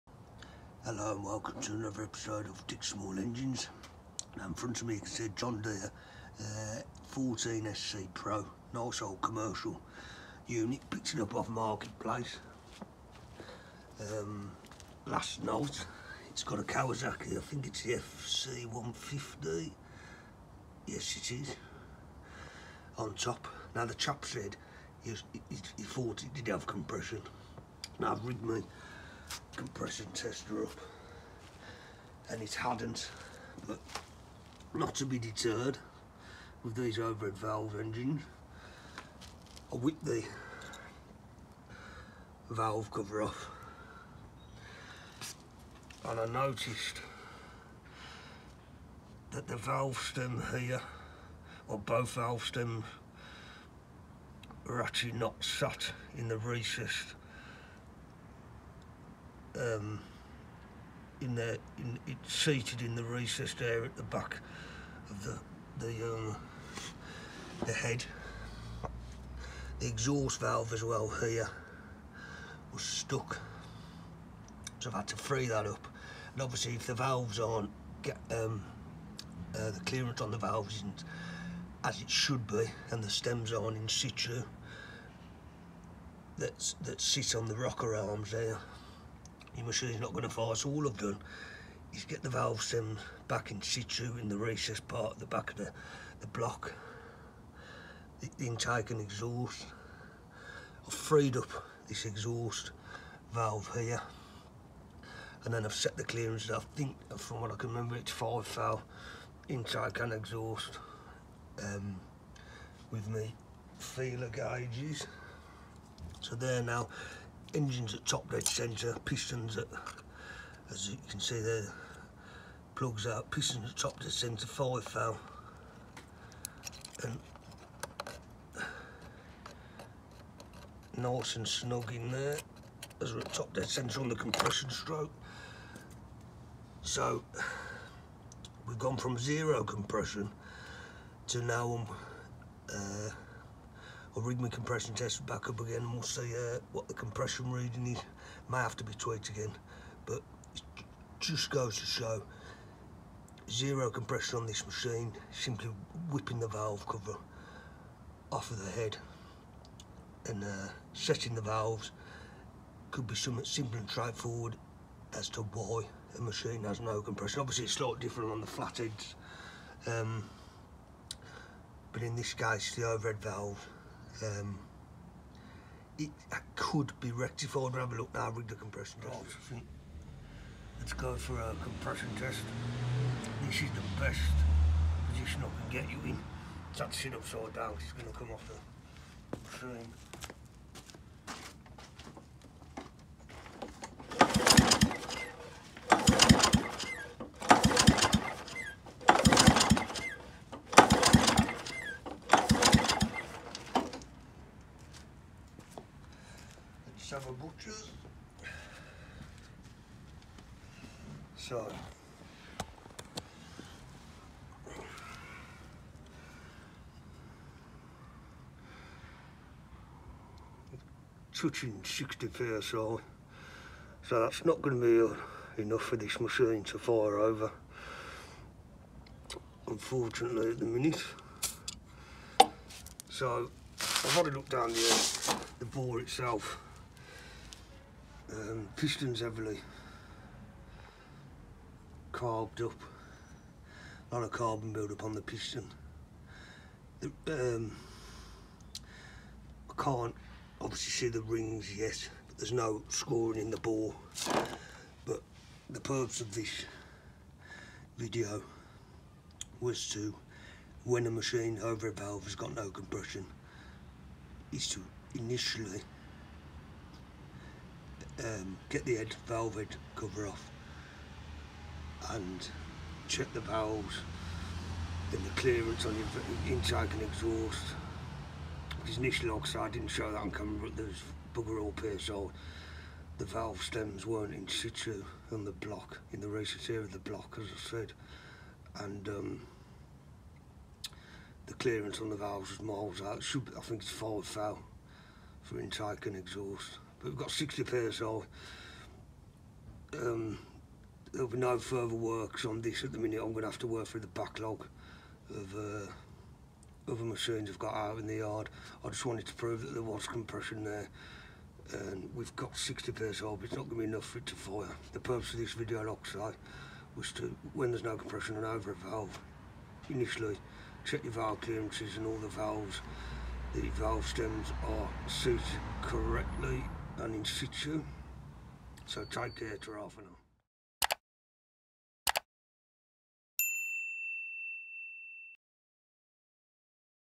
Kawasaki FC150V- No compression. Check this first.
Kawasaki FC150V engine. No compression. Here I look at the above engine with zero compression following an initial compression test. Then a first port of call for any OHV engine without compression (in my humble opinion) in inspecting valve clearances & valve stem configurations which could be a factor in low compression readings.